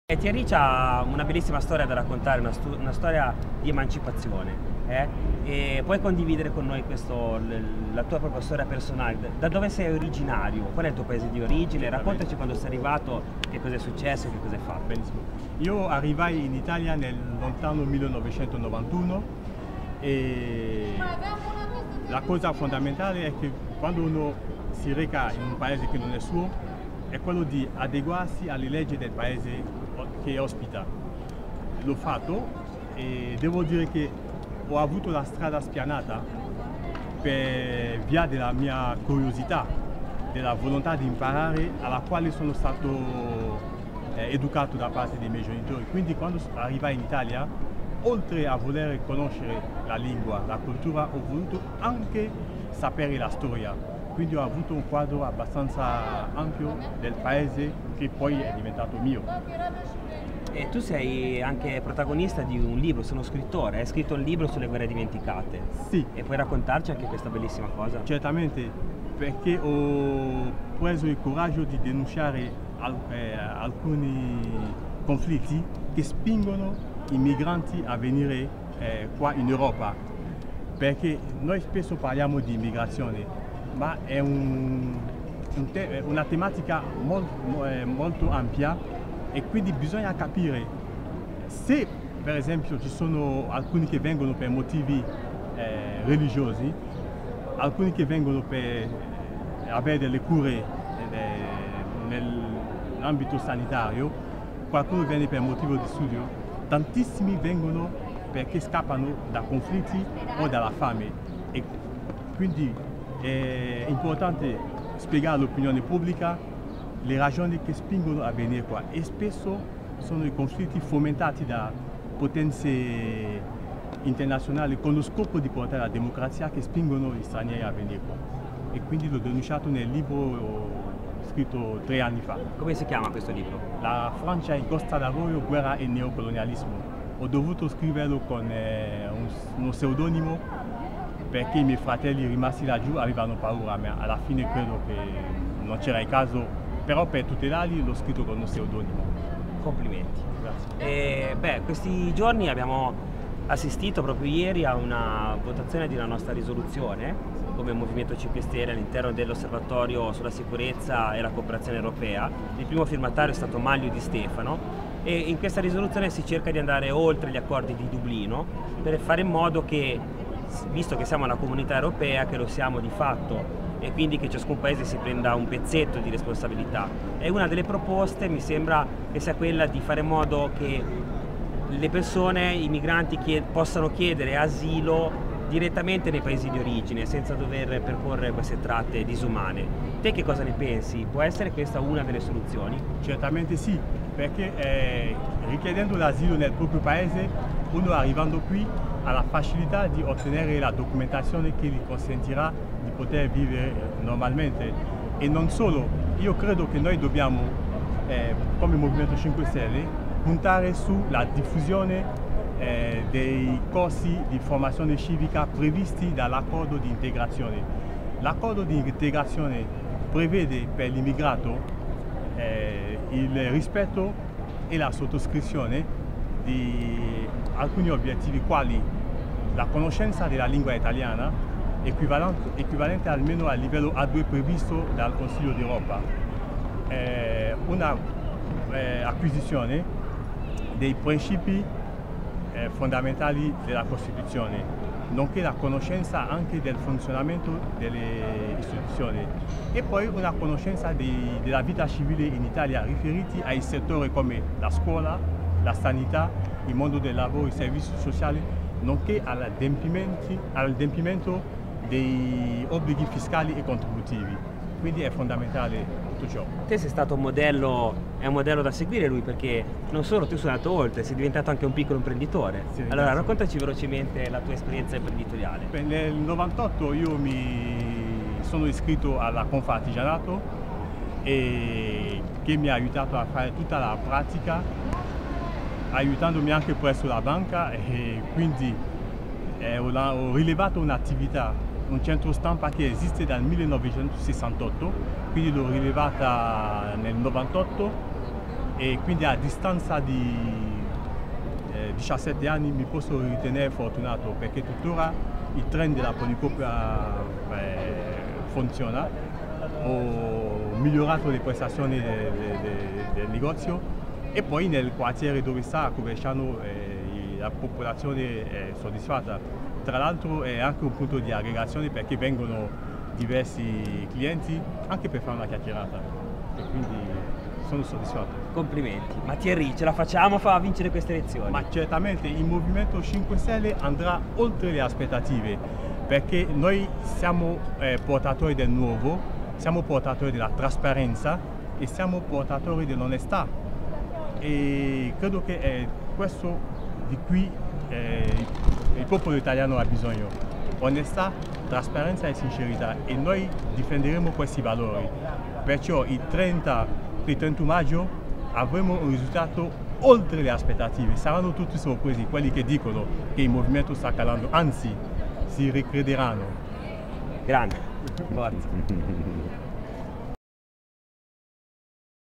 Giacomo Giannarelli e Thierry Abdon Avi - Immigrazione
Incontriamo Thierry Abdon Avi, candidato consigliere del Movimento 5 Stelle, per parlare della sua storia di buona integrazione.
L'immigrazione è tema serio e noi lo affrontiamo ad ogni livello istituzionale.